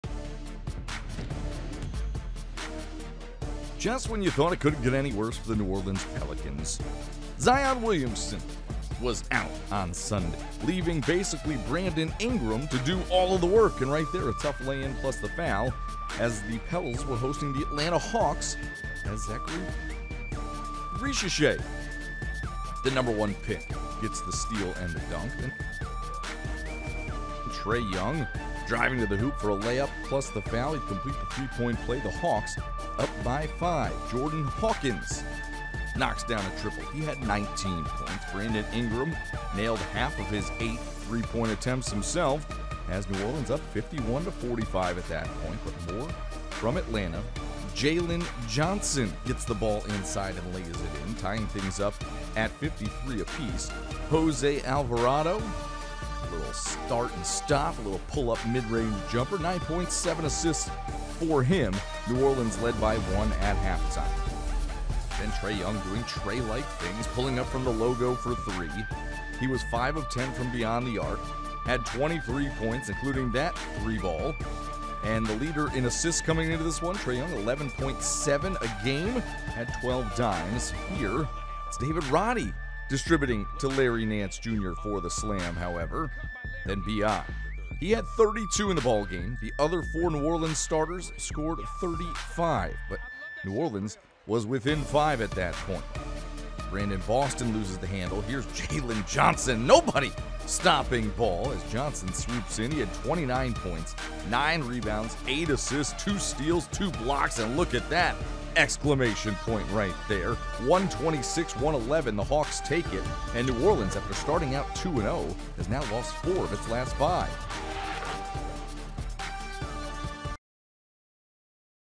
Game Recap: Hawks 126, Pelicans 111
The Hawks defeat the Pelicans, 126-111.  Jalen Johnson led the way with 29 points, nine rebounds and eight assists for the Hawks, while Trae Young added 23 points and 12 assists in the victory.  Brandon Ingram tallied 32 points, seven rebounds and four 3pt. FG for the Pelicans in the losing effort.  The Hawks improve to 3-4 on the season, while the Pelicans fall to 3-4